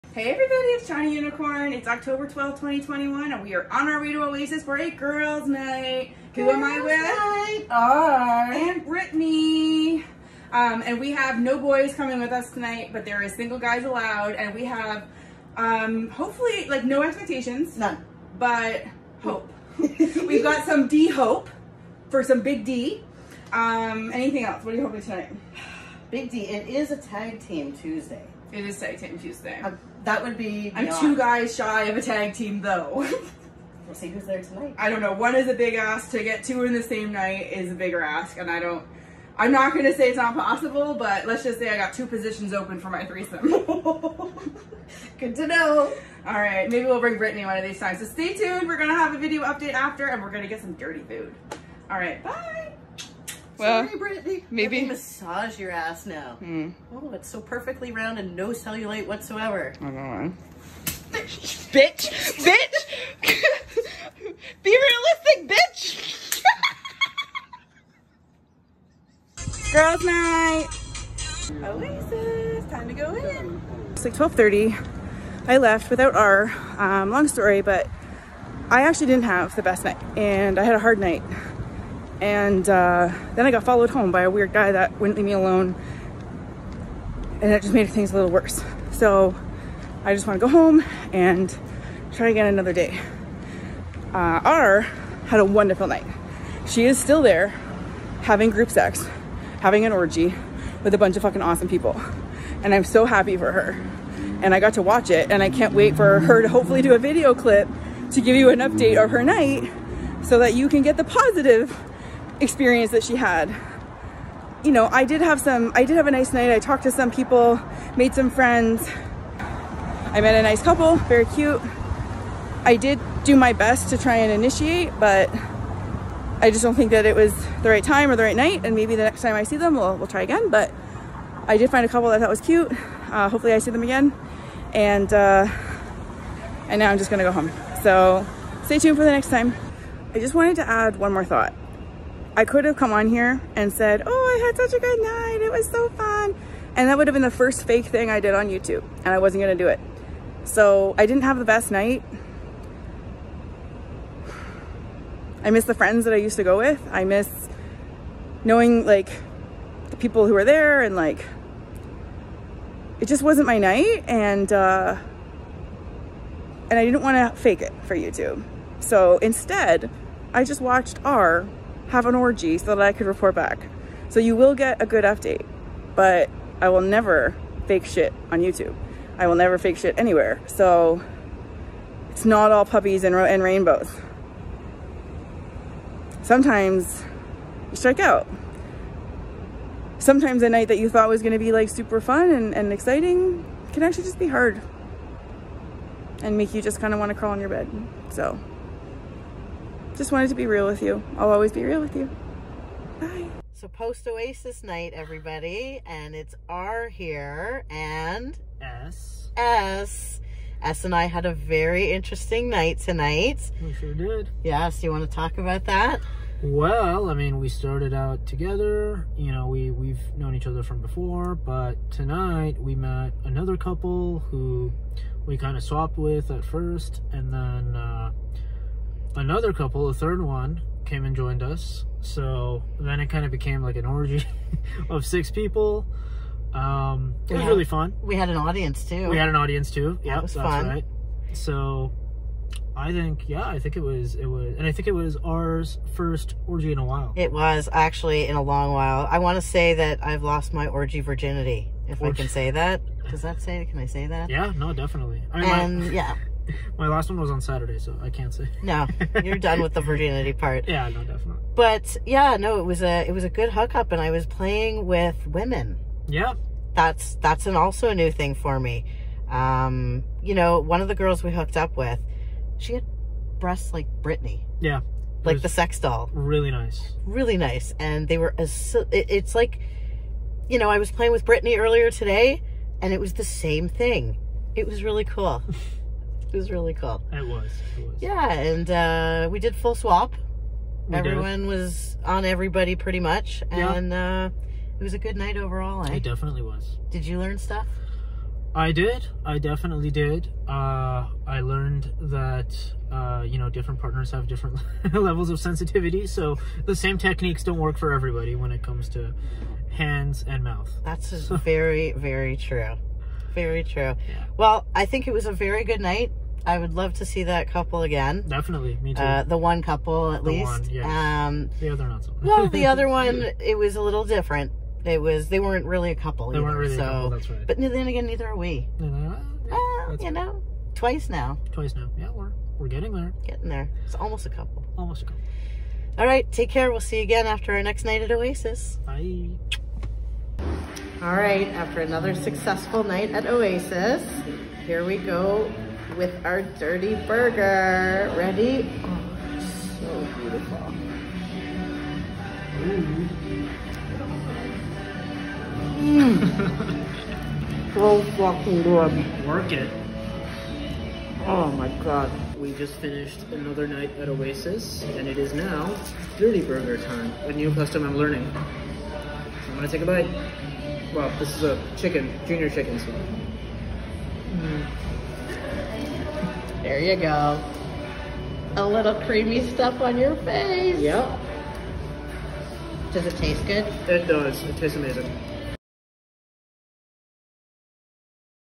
Girls night at Oasis with R on “Tag Team Tuesday” 😳😜😈 STORYTIME
Our first girls night to Oasis in two weeks!  It started out great for both of us but I ended up leaving early (not my best night… for a few reasons). However “R” had QUITE the night and she spills ALL the details!!